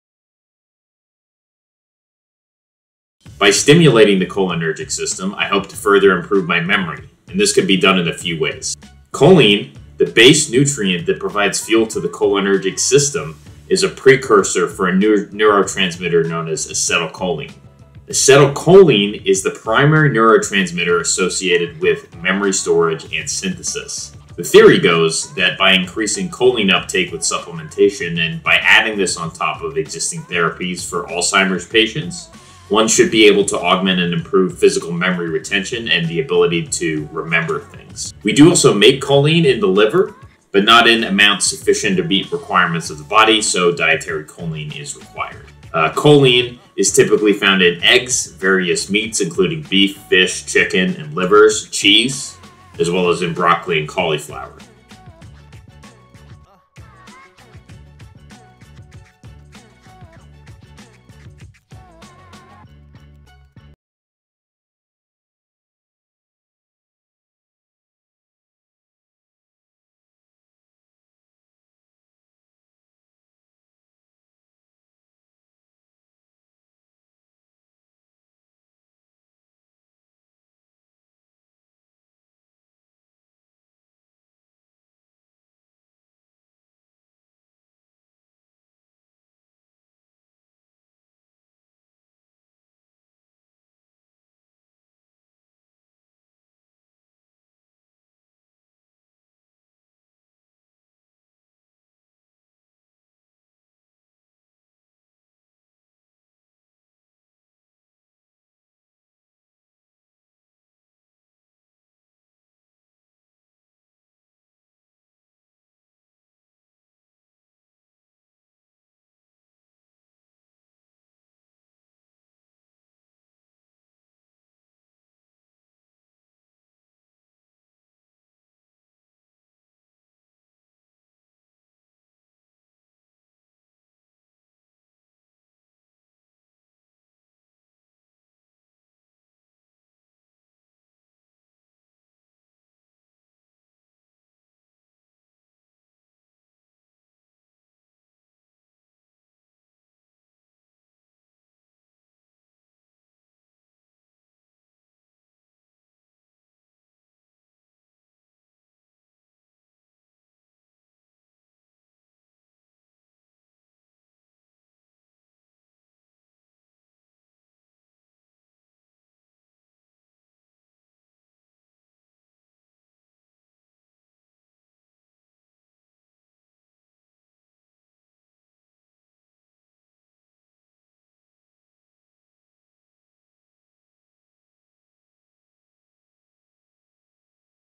Short: Choline and Memory Basics
What is choline and what does it have to do with your memory? Can you biohack using choline and acetylcholine to boost your memory?

Details here!